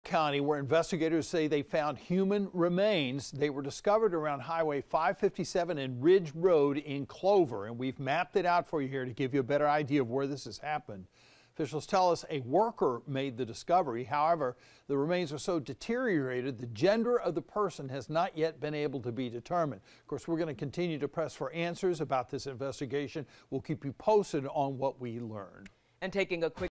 Sheriff: Human remains found in York County near Hwy. 557
Officials state that workers found the remains on Hwy. 577 and Ridge Road.